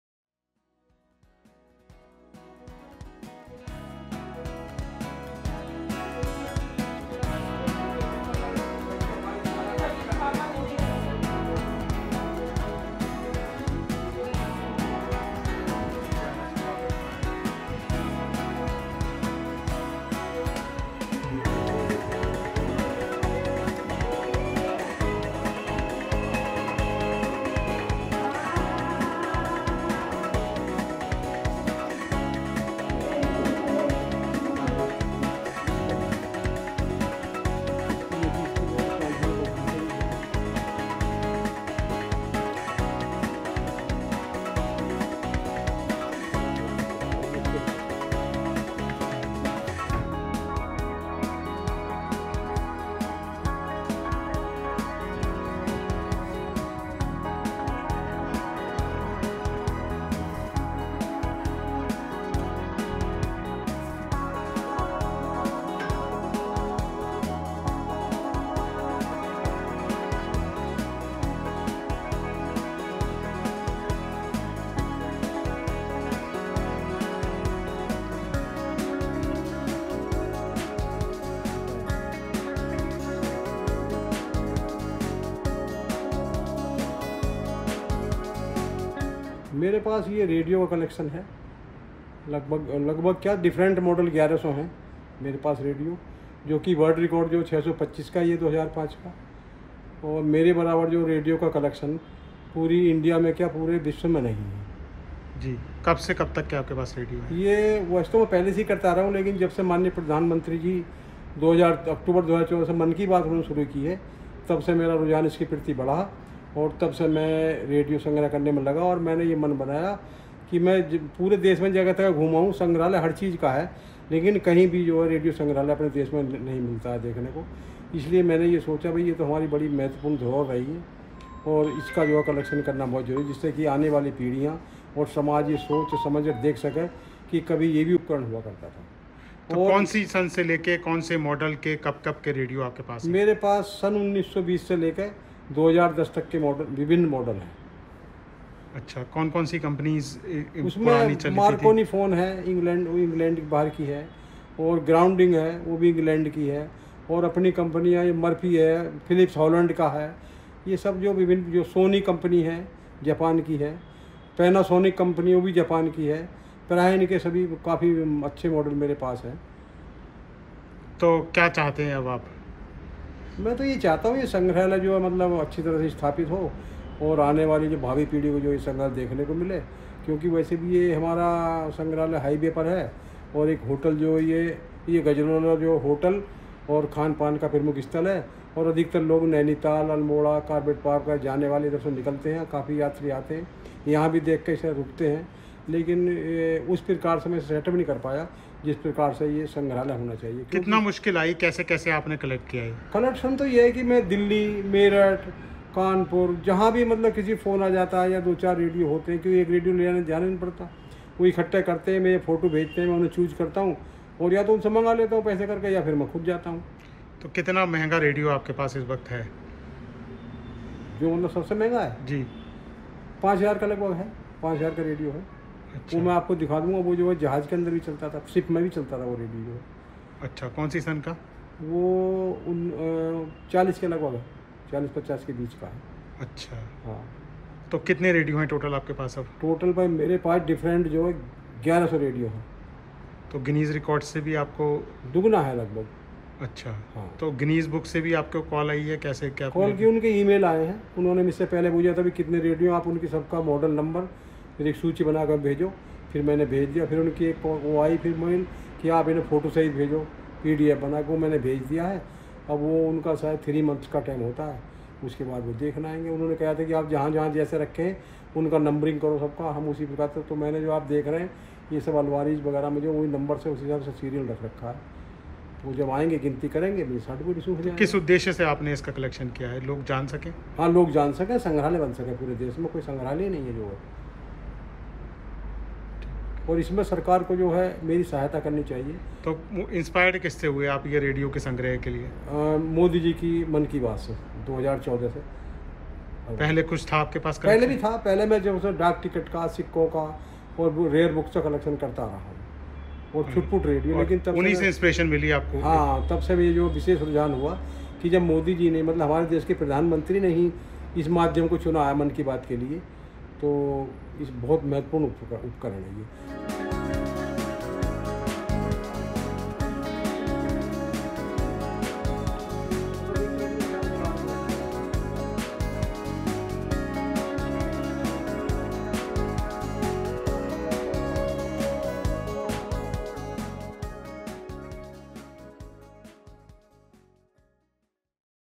PM Modi के Mann Ki Baat से प्रेरित होकर यूपी के शख्स ने जुटाए 1,100 Radio Sets
उत्तर प्रदेश (Uttar Pradesh) के अमरोहा (Amroha) के एक व्यक्ति ने प्रधानमंत्री नरेंद्र मोदी के "मन की बात" से प्रेरित होकर 1,100 रेडियो सेट एकत्र किए. राम सिंह, जो एक सेवानिवृत्त सरकारी कर्मचारी हैं, उनके संग्रह में विभिन्न आकार और आकार के रेडियो हैं.